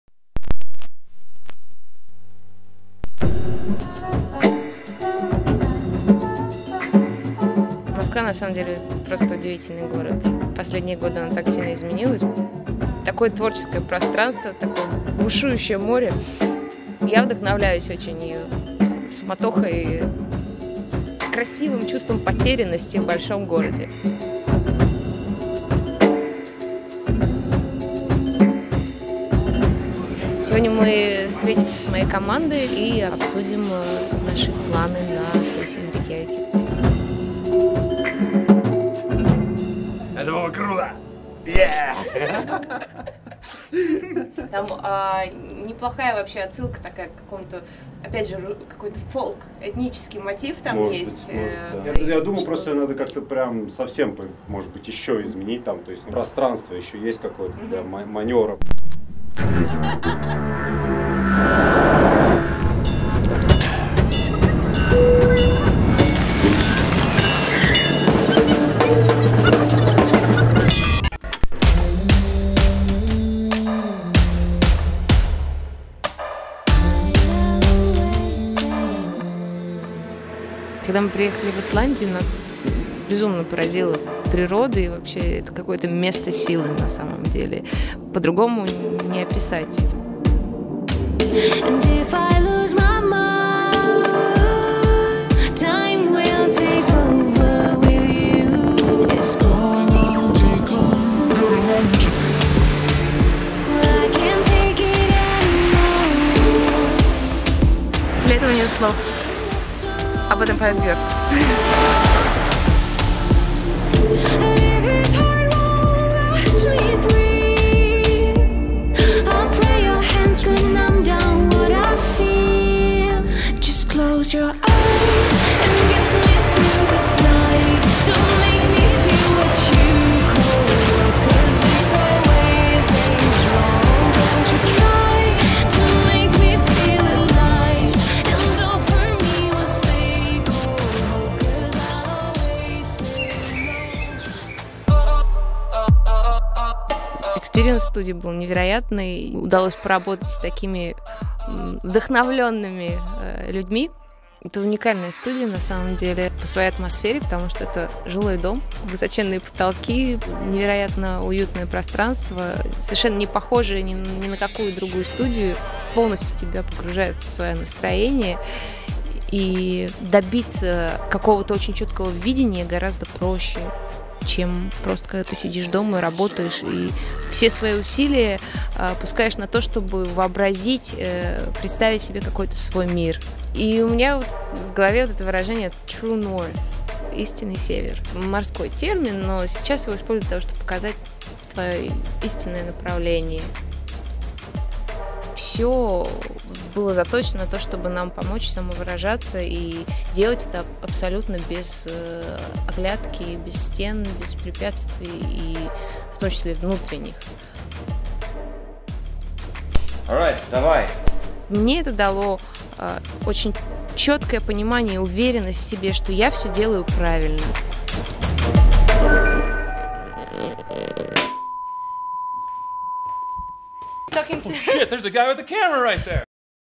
Converse Rubber Tracks in Iceland: Noonwraith (ENG subs)
In September 2015, Converse Rubber Tracks was giving the keys to 12 of the world's best recording studios to 84 unsigned bands. Out of 9,000 applications Noonwraith was the only Russian music act to have won a 2-day recording session at Greenhouse Studios in Reykjavik, Iceland: a legendary place where Bjork, Damien Rice, The xx and so many other music sorcerers worked their magic. This was our adventure!

A great many thanks go to the team of Converse Russia for their incredible support and making this video possible; to our studio engineer Justin Douglas for being such a master of his craft and an unbelievably awesome guy. To the amazing electronic sound producer Haarps for saving the day (check his rad tunes and Alexandr Dorinov for capturing this magical experience and turning it into this video!